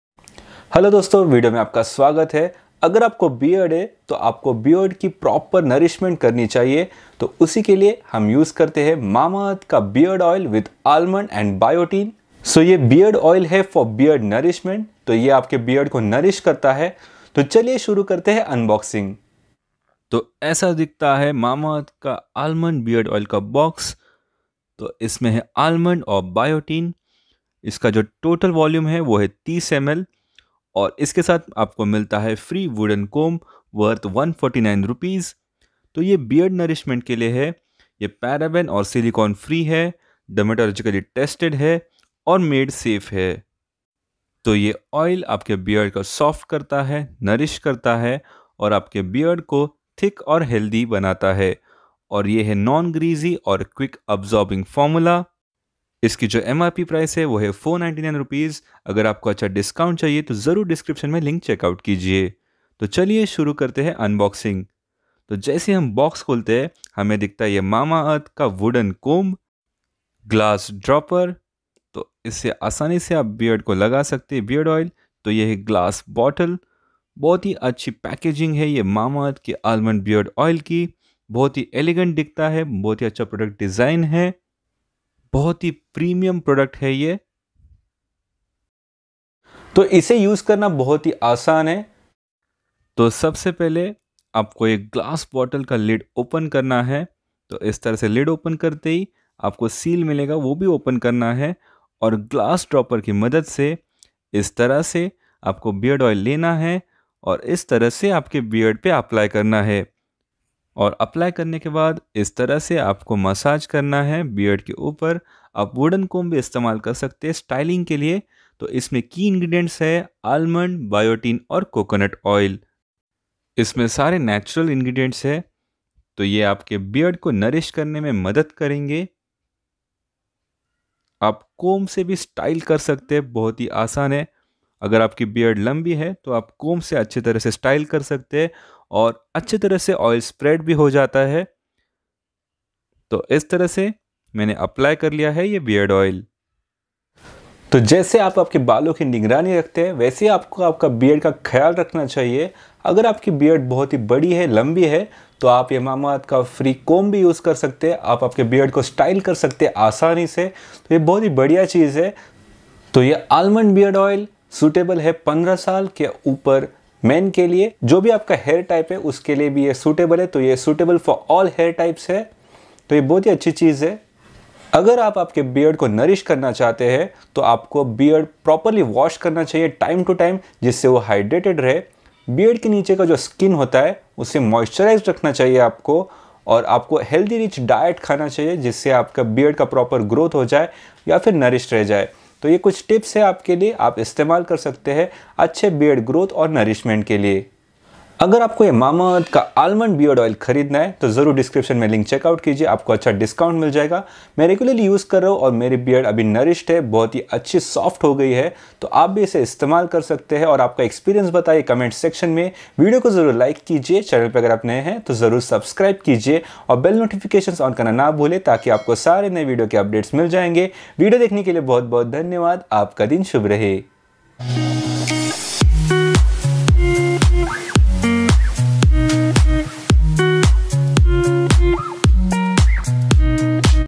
Mamaearth Almond Beard Oil for Men | For Fast growth with Almond & Biotin for Beard Nourishment
Get to know about Mamaearth Almond Beard Oil. This is Mamaearth Almond Beard Oil Review, we have used it and shared our experience in this video.